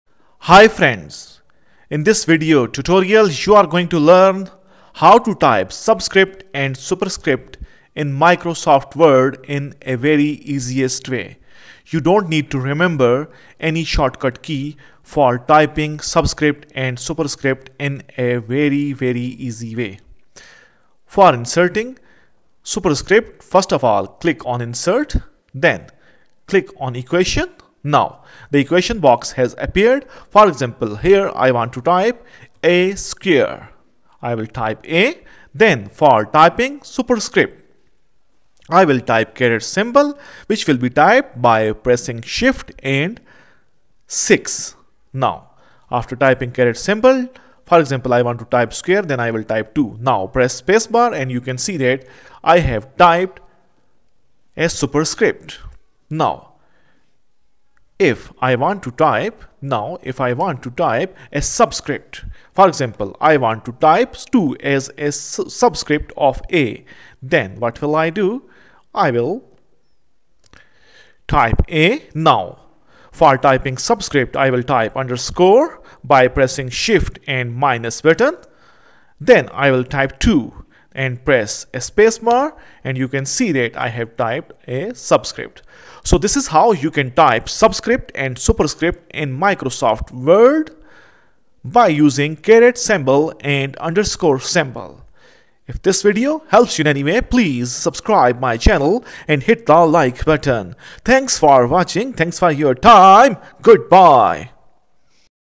Easiest Way to Type Subscripts and Superscript in MS Word | Type Sub and super Script , No Shortcut
This video describes how to type subscripts and superscripts in Microsoft Word in the easiest way. This video shows a very easy way to type subscript and superscript in Microsoft word without using a shortcut key. After watching this video you will be able to type subscript and superscript in Microsoft Word.